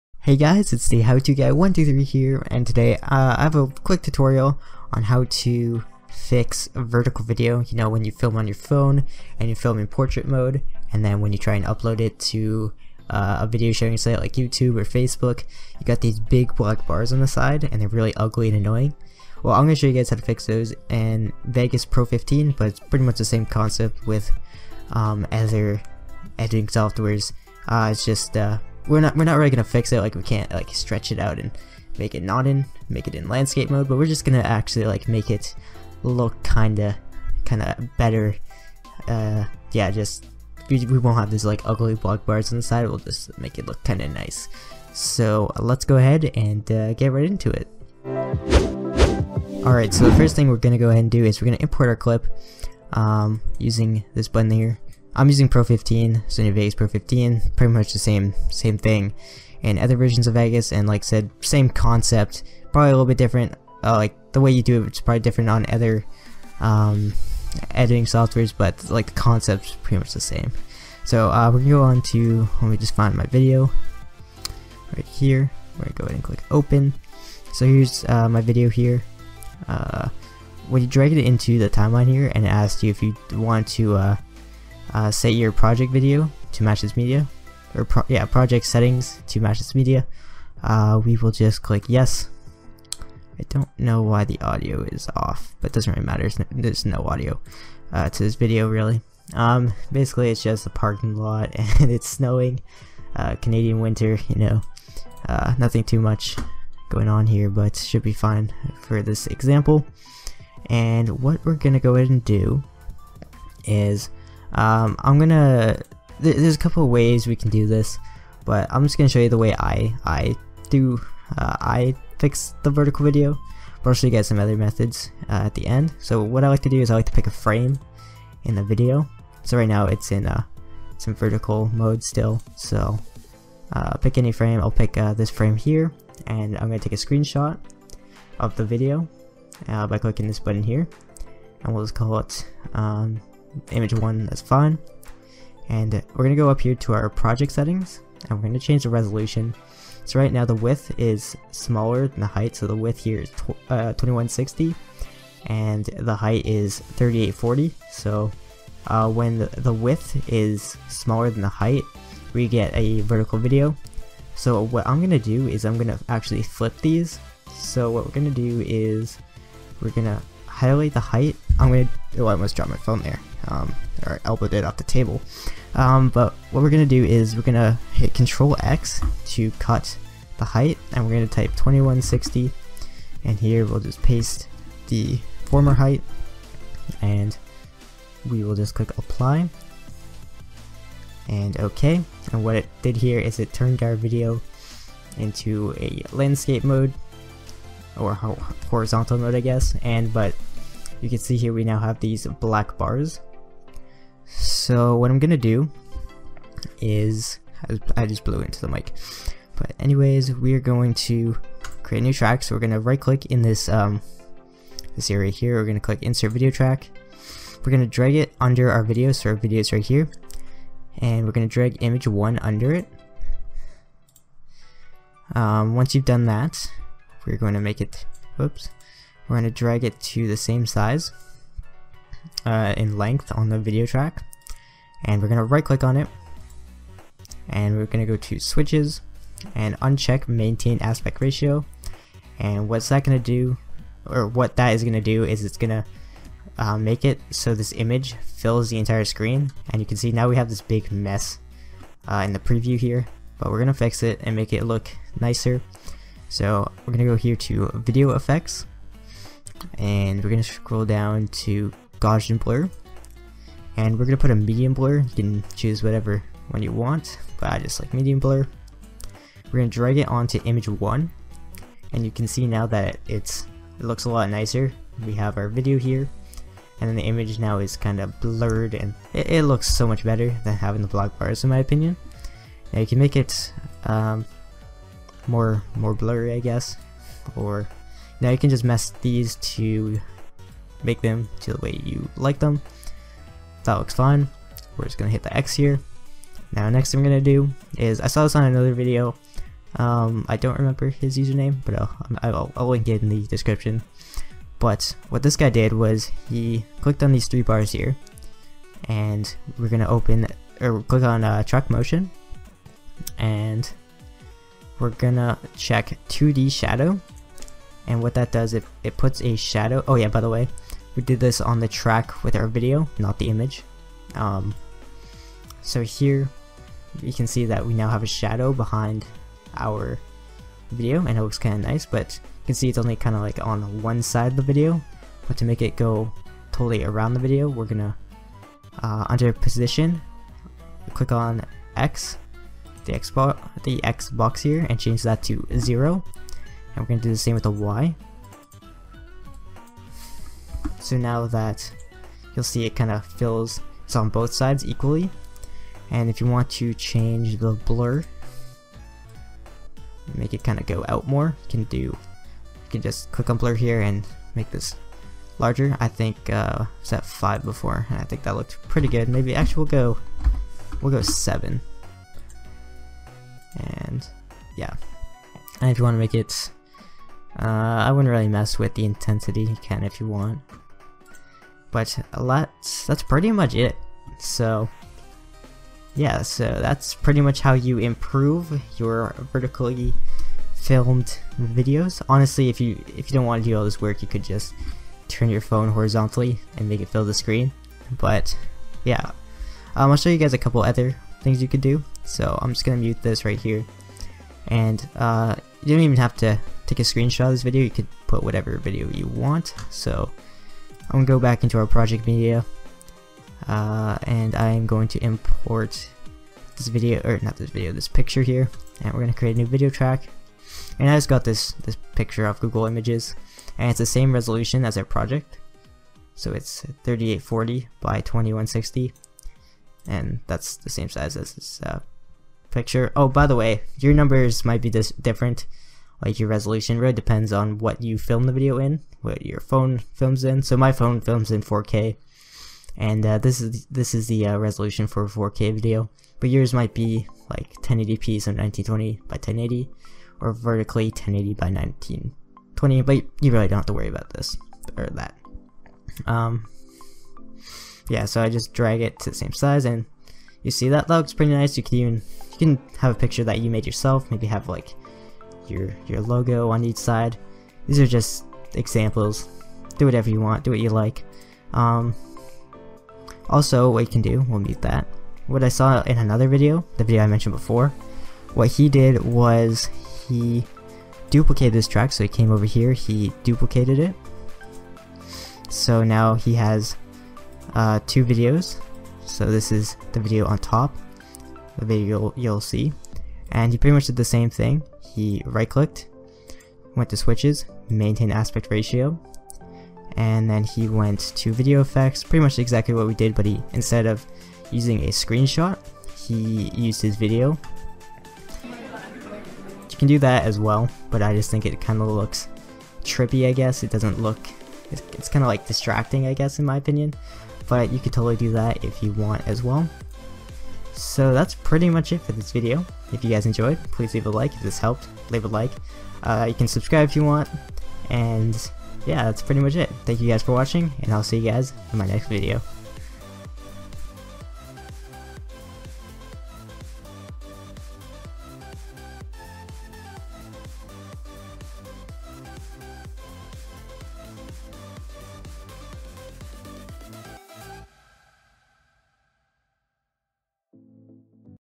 How To Improve/Fix Vertical Videos In Sony Vegas
Tutorial Starts at 0:50.

How To Improve/Fix Vertical Videos In Sony Vegas. This is useful for when you record videos on your phone vertically, so when you upload it to a video sharing site you don't get the ugly black bars on the side.